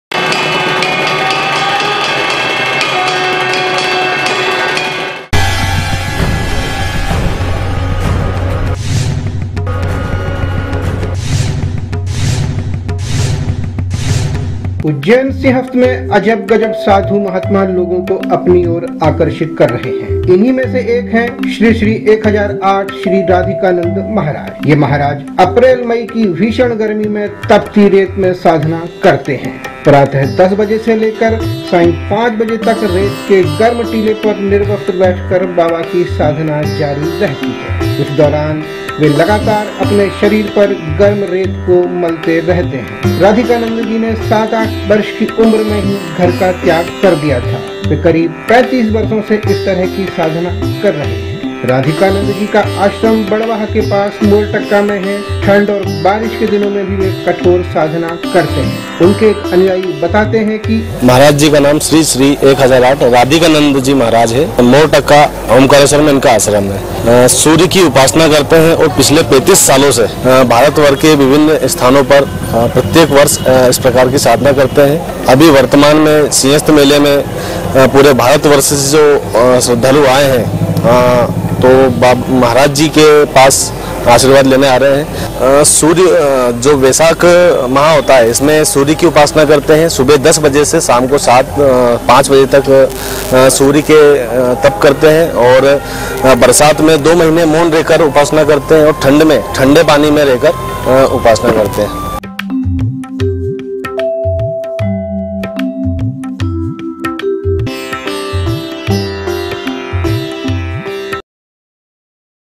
तपती रेत में निर्वस्त्र साधना
उज्जैन सिंहस्थ में राधिकानंदजी महाराज लोगों के आकर्षण का केन्द्र हैं, जो भरी गर्मी में तपती रेत पर निर्वस्त्र साधना करते हैं।